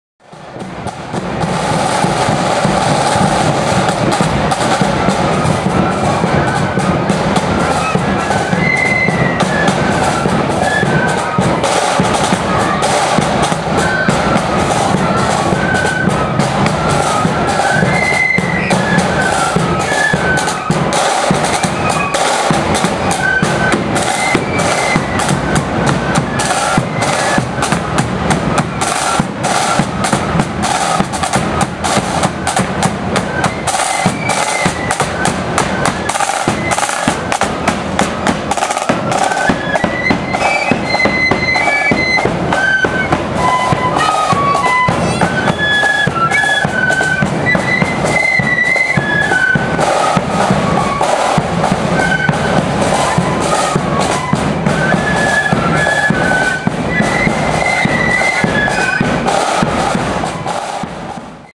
Robert Graham Memorial @ Pride of the Maine 07/08/14
Robert Graham Memorial On the March in Ballymena at Pride of the Maine's Annual Band Parade